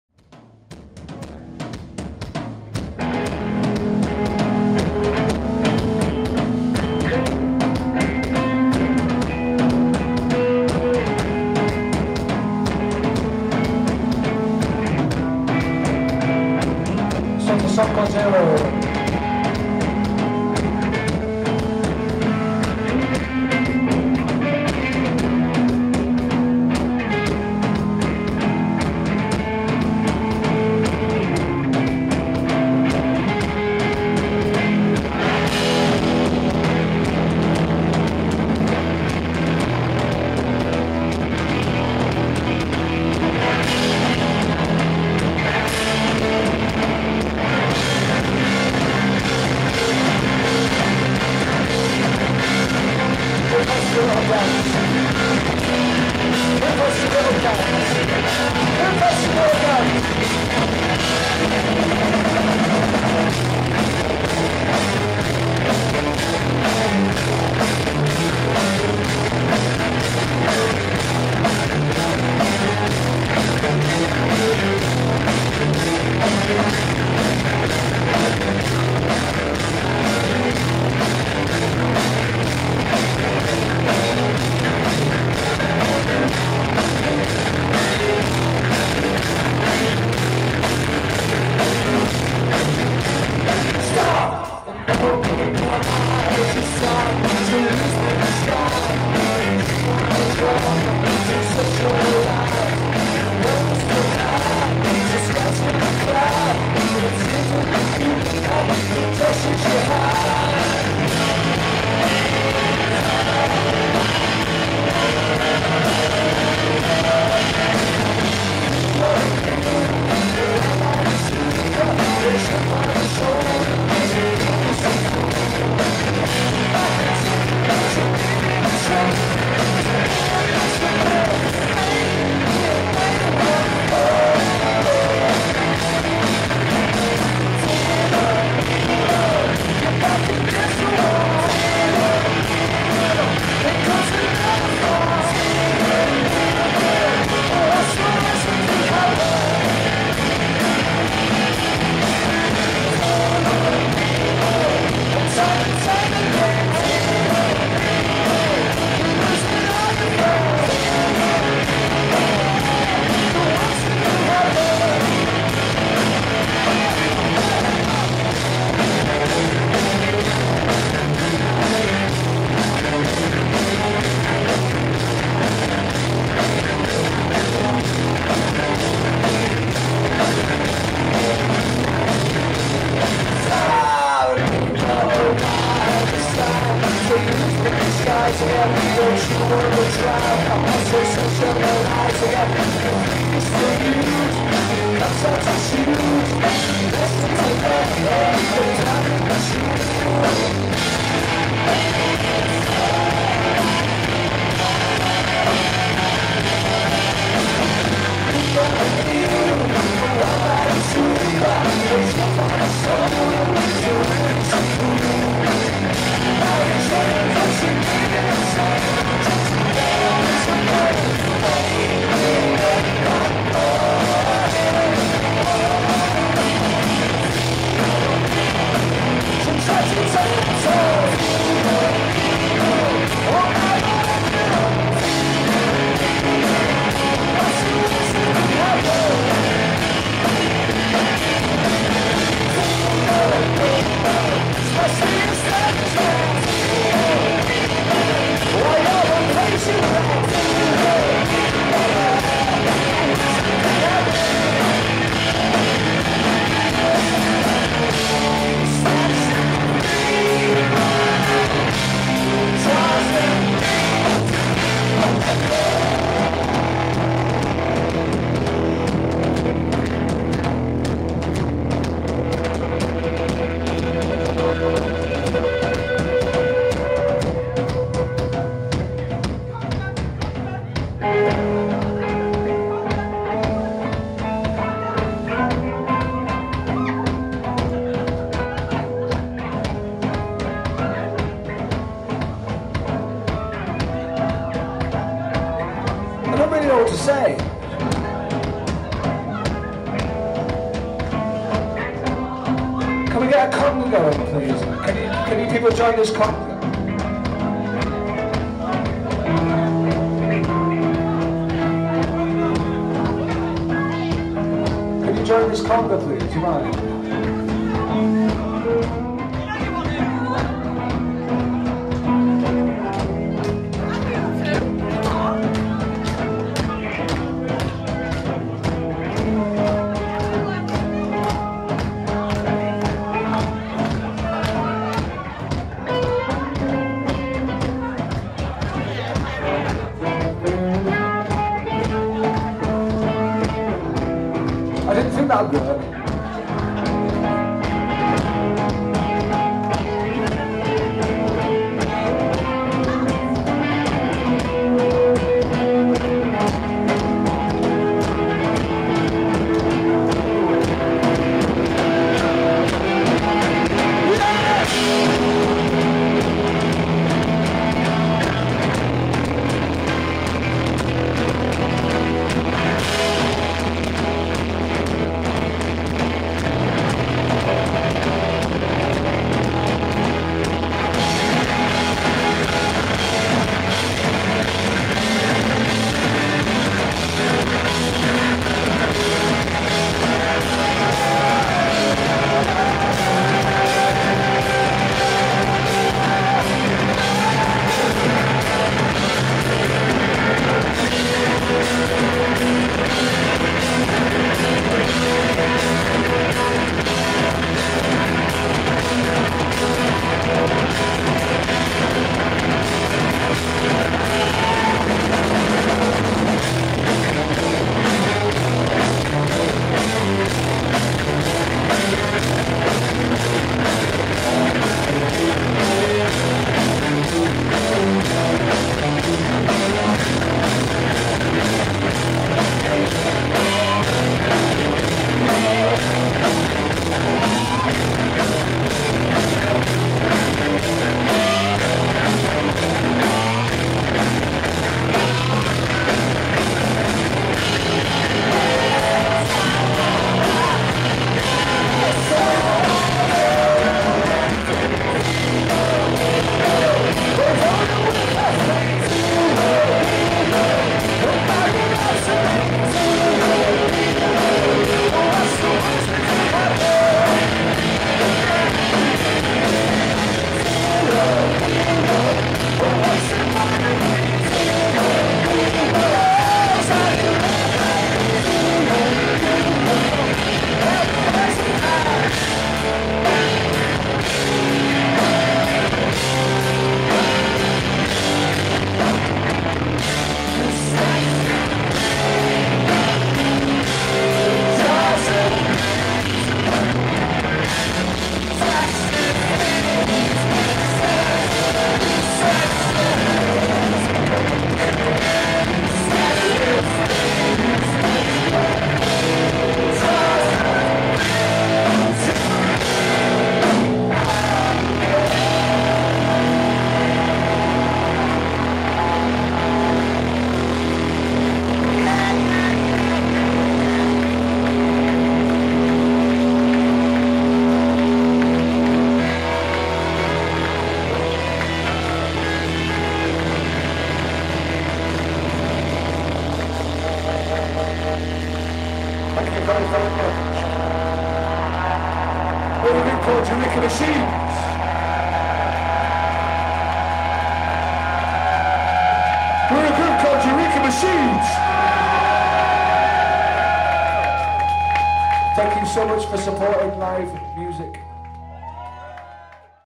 Eureka Machines - Can We Start A Conga?
Eureka Machines play The Asylum 2 in Birmingham and a conga breaks out.